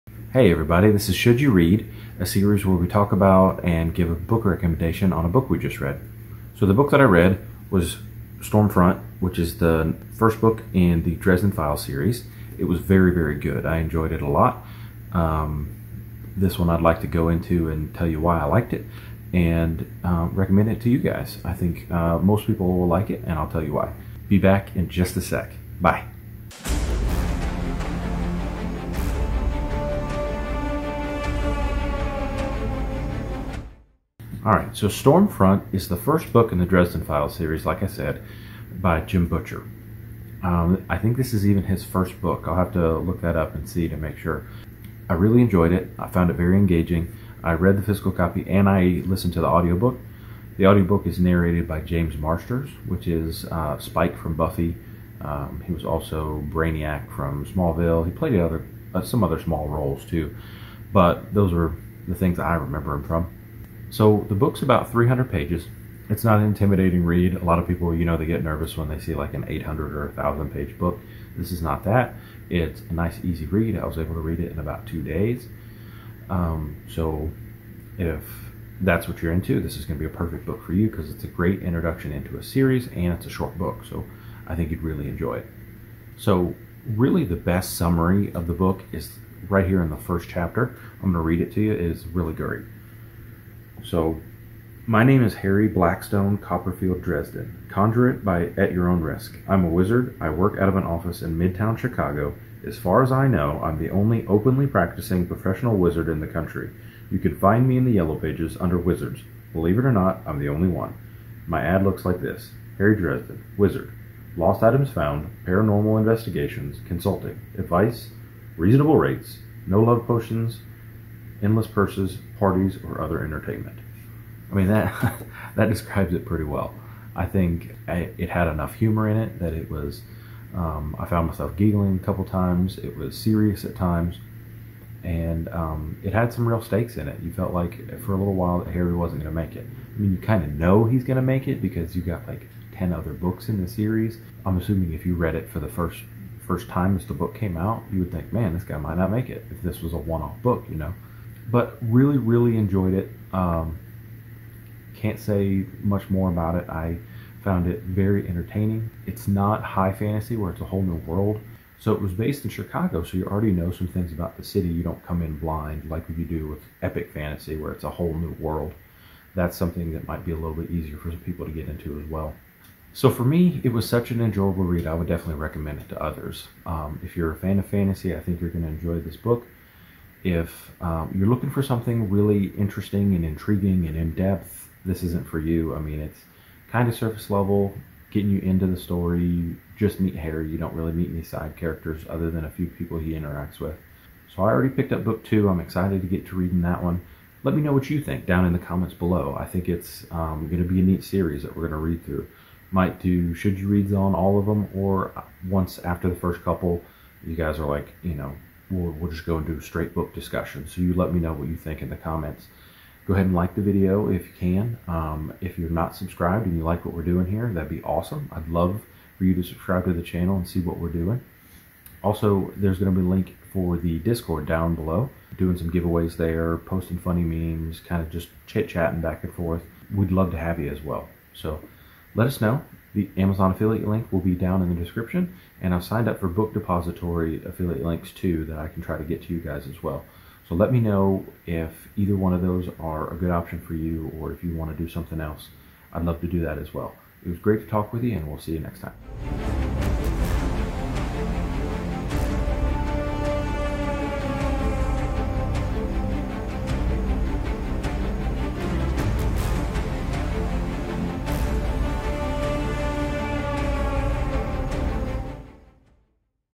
Storm Front
Storm Front by Jim Butcher. Should you read? Lets discus and let me know what you think.